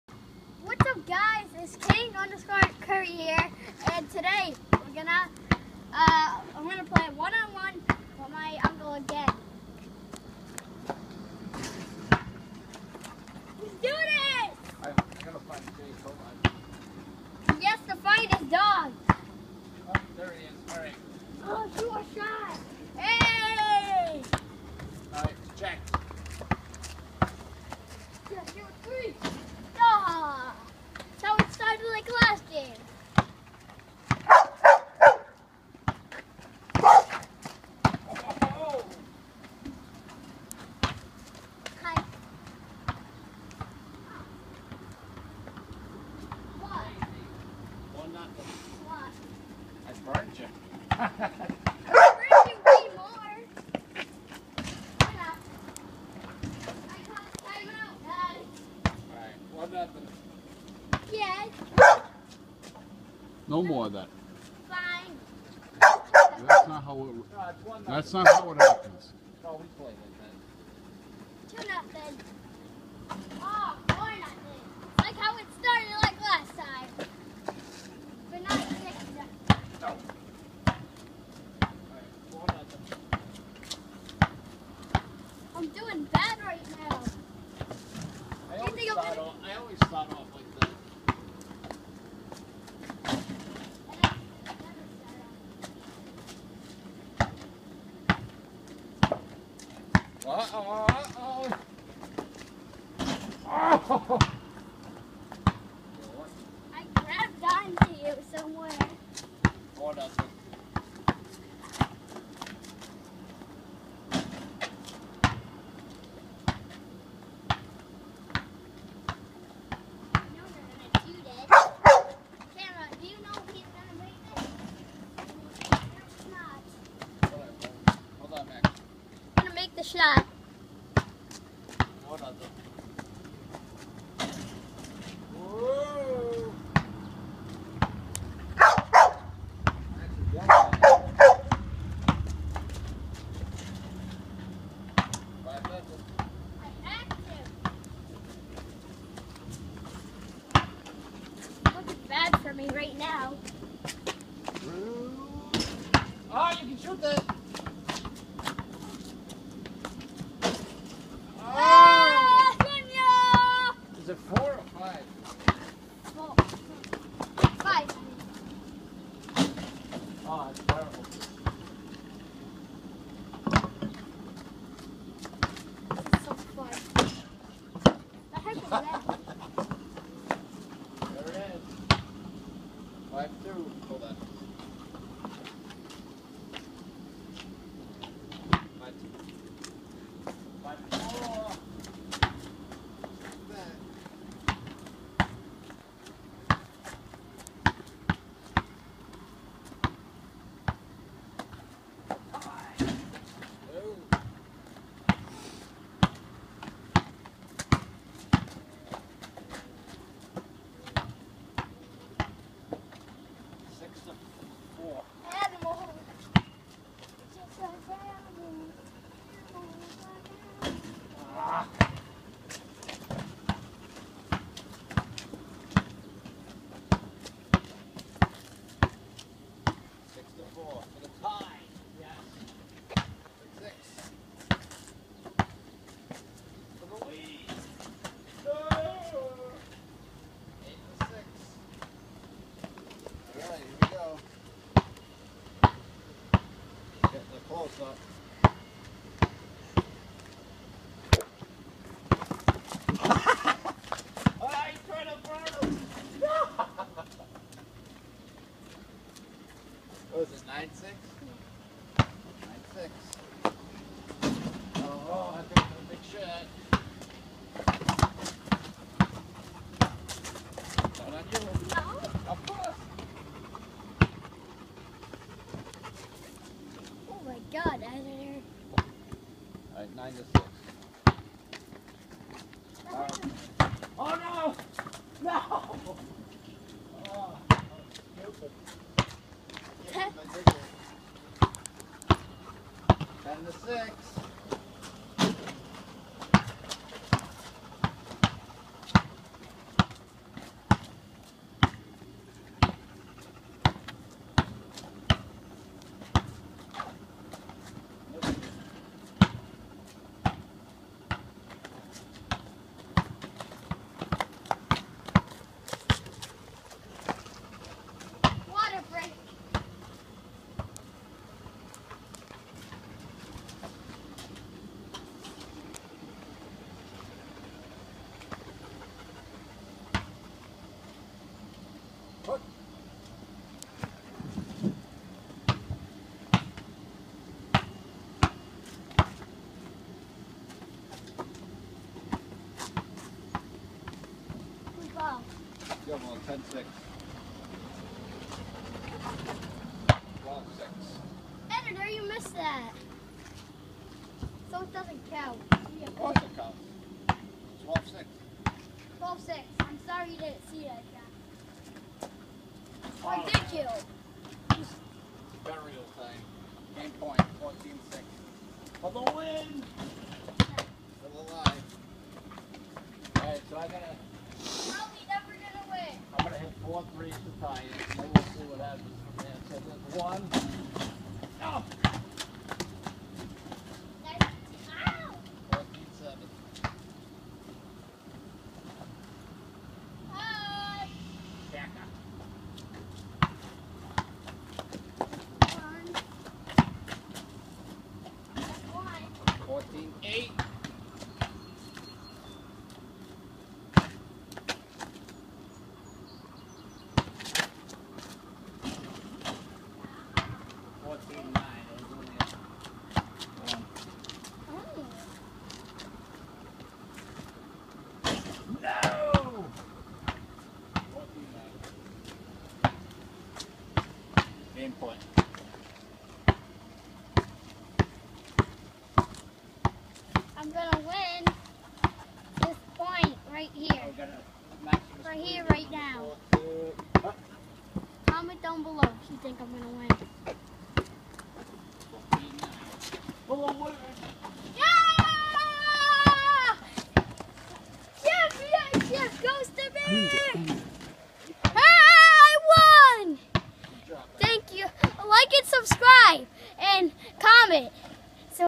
4th of July 1 on 1 Basketball 🏀 Special with Max The Labrador Retriever
4th of July One on One!